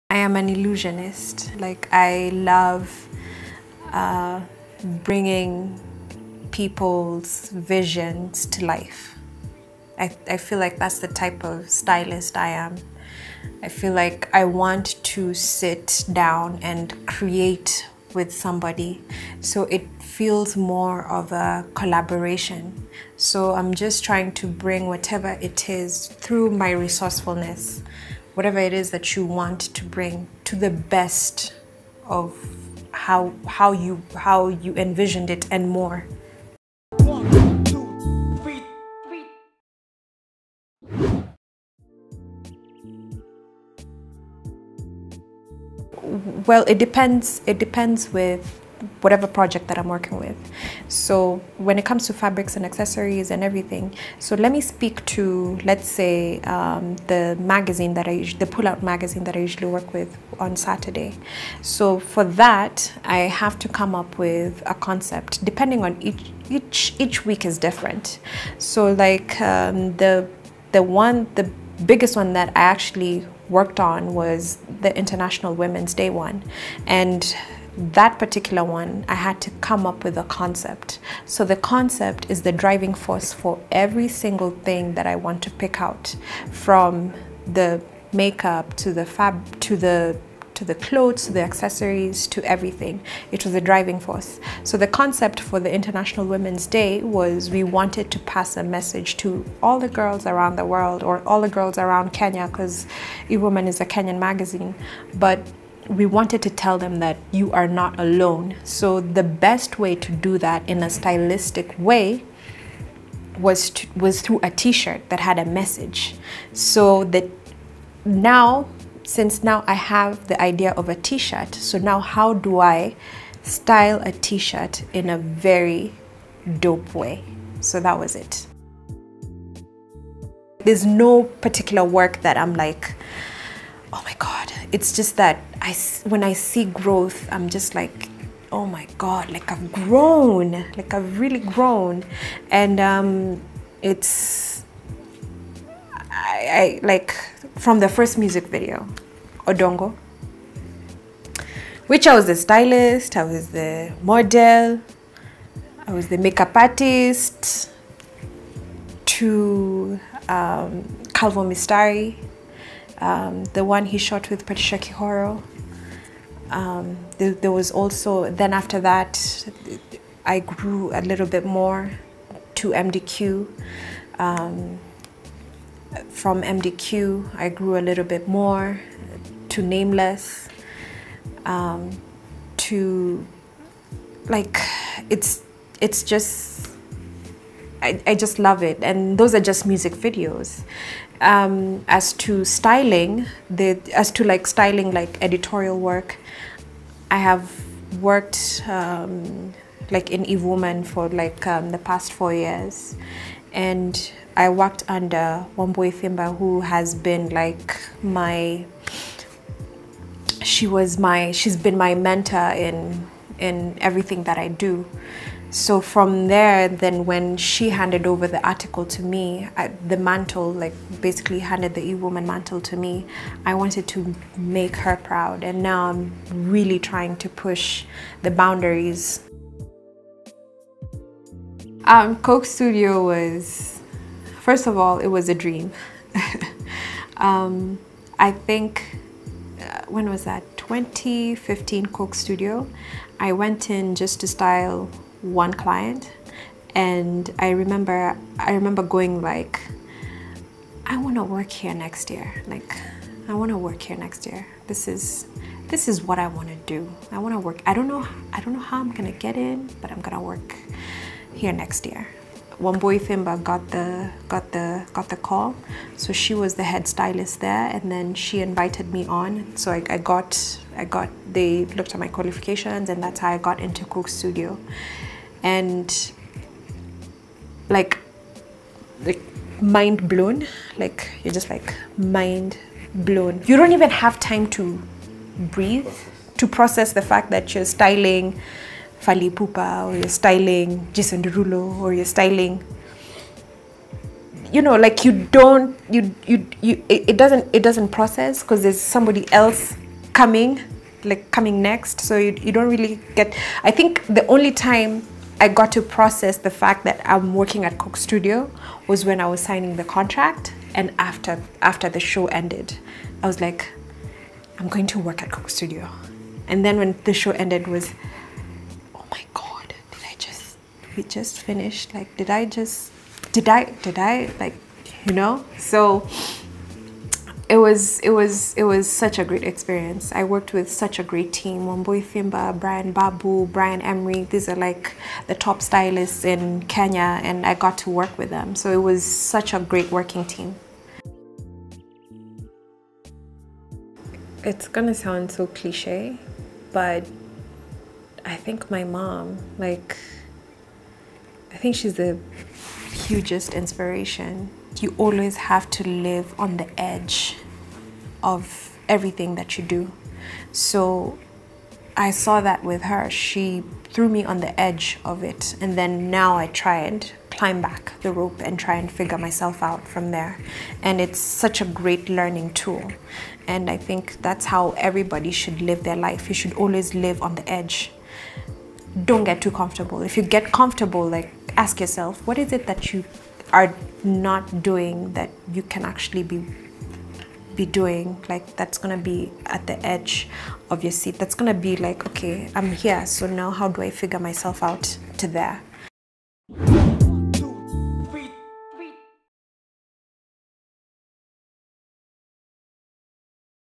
123 talks to Lucy Robi [+IN MY SPACE+] | 123Sketches
Robi talks styling at COKE STUDIO, her favorite work,EVE WOMAN Magazine journey and much more.